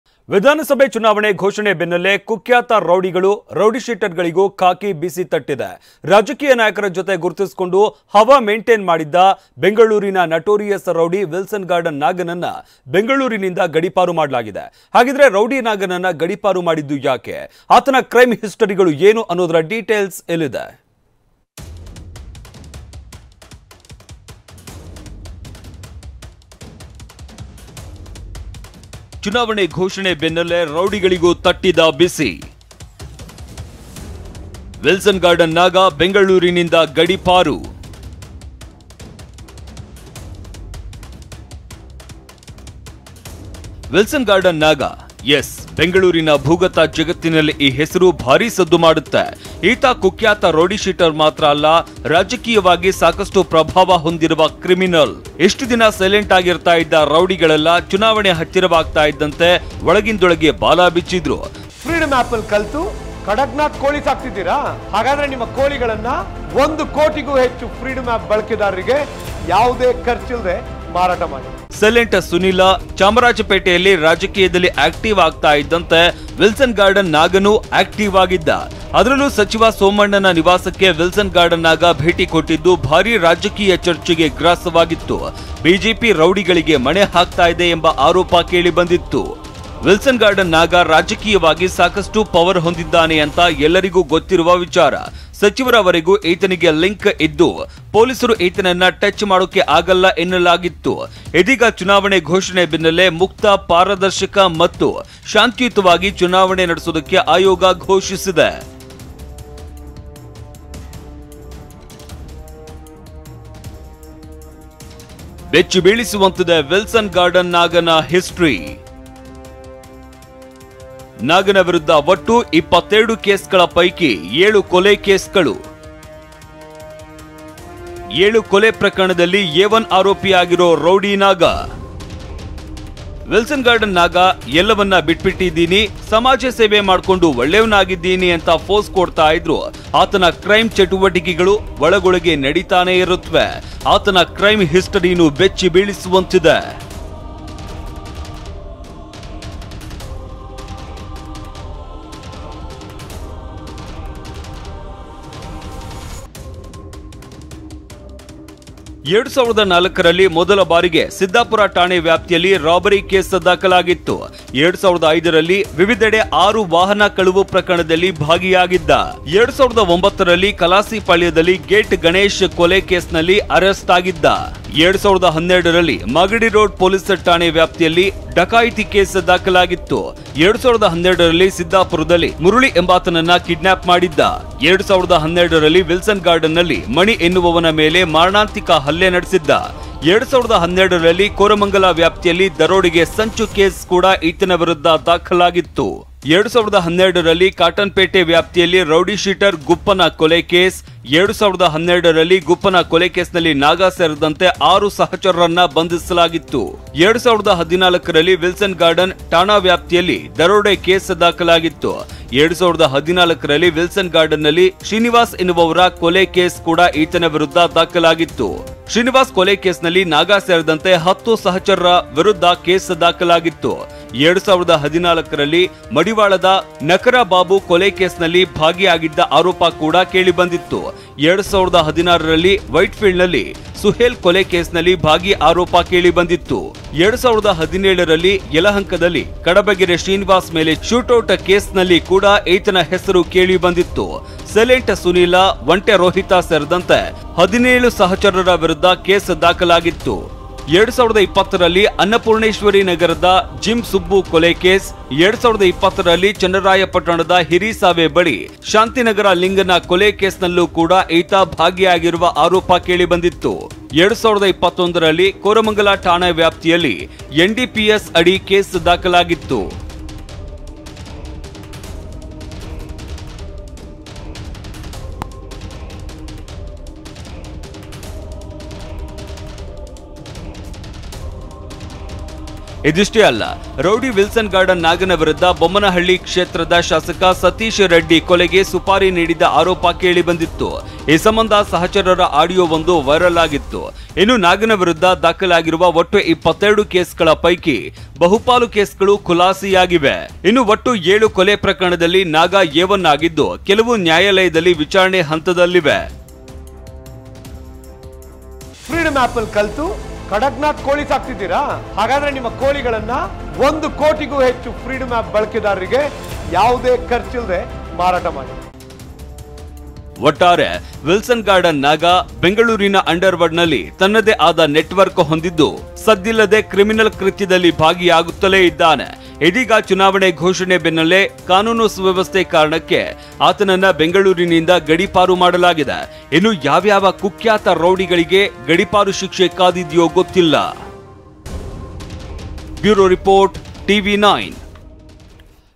Rowdy Sheeter Wilson Garden Naga Exiled From Bengaluru Ahead Of Elections
Download ffreedom app and apply coupon “TV9” to avail Rs 3000 scholarship instantly

ಚುನಾವಣೆ ಅನೌನ್ಸ್ ಬೆನ್ನಲ್ಲೇ ಖಾಕಿ ಅಲರ್ಟ್​- ವಿಲ್ಸನ್ ಗಾರ್ಡನ್ ನಾಗ ಗಡಿಪಾರು-ಬಾಂಡ್ ಬರೆಸಿಕೊಂಡು ವಾರ್ನಿಂಗ್